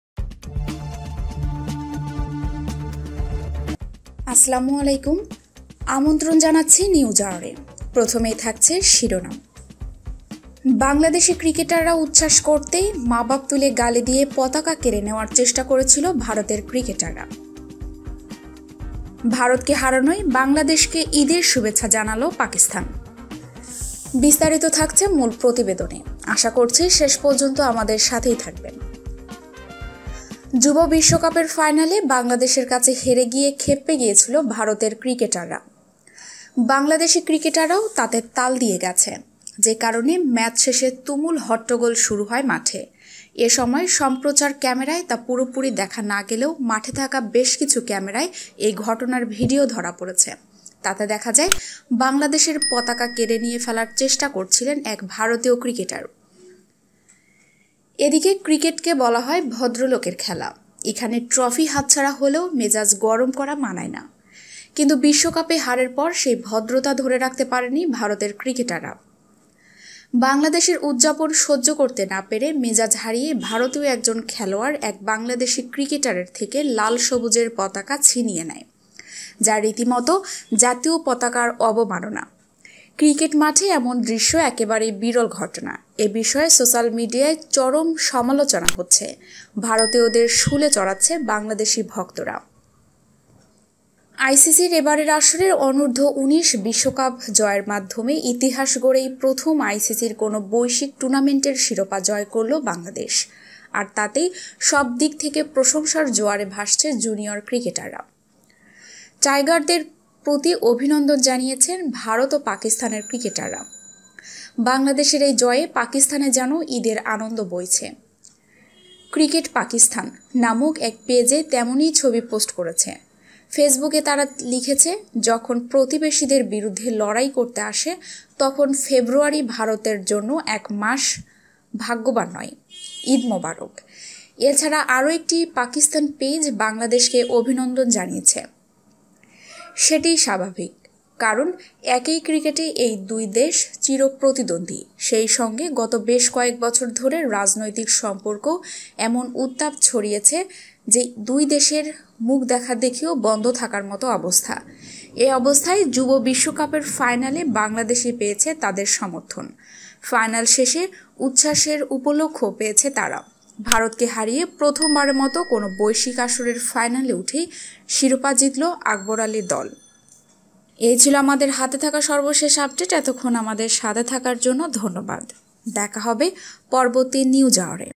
ভারতকে হারানোয় বাংলাদেশকে ঈদের শুভেচ্ছা জানাল পাকিস্তান | bangladesh vs india u19 final 2020
মা-বাপ তুলে গালি দিয়ে, পতাকা কেড়ে নেয়ার চেষ্টা করেছিল ভারতের ক্রিকেটাররা